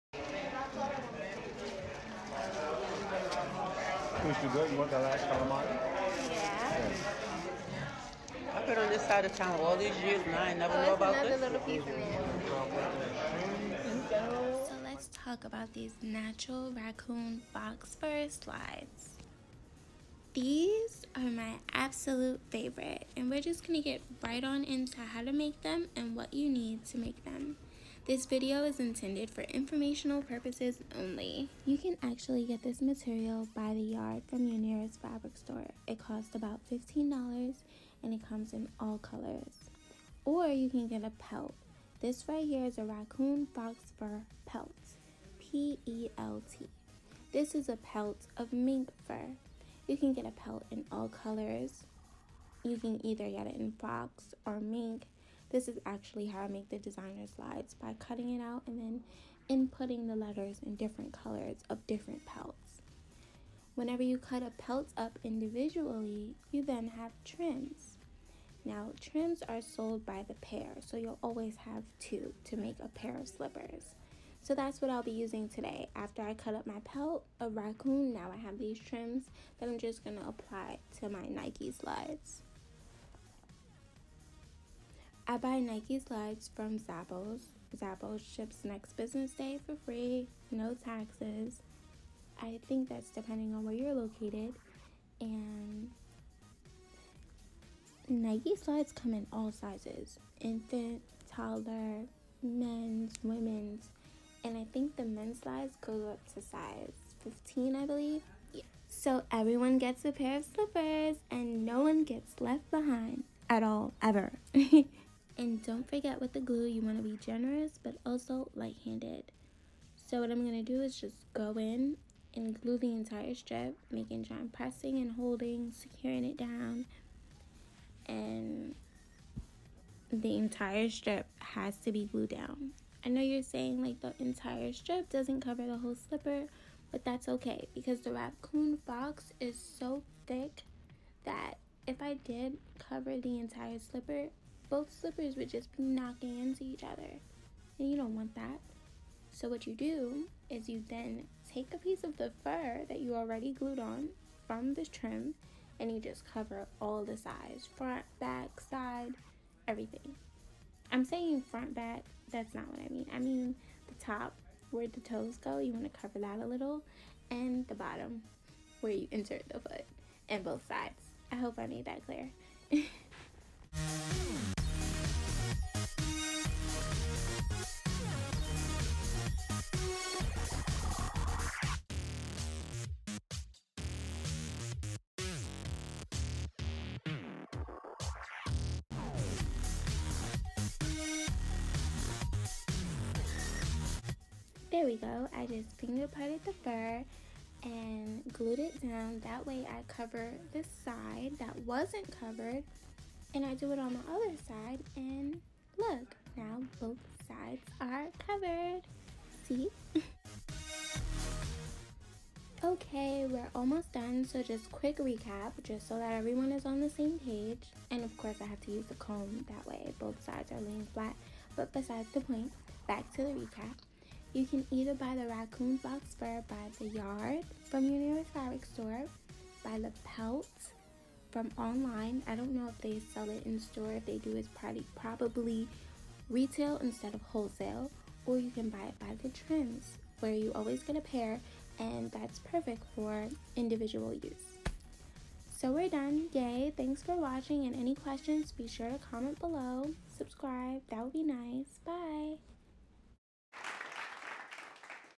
DIY: Raccoon Fox Fur Slides
Hey this is a short video on how to & what you’ll need.

All things can be searched:

Raccoon fox fur strips
Raccoon fox fur trim
The fox fur trim can either get you the material by the yard or the cut up pieces from the pelt.
Raccoon fox fur pelt or raccoon fox fur skin

You can search all of these terms on google & see what you find.

If you’d like to buy the pieces from me a pair of trims

Buy the glue:

Personal IG: @GoddessPucci
Business IG: @sugarnspice.life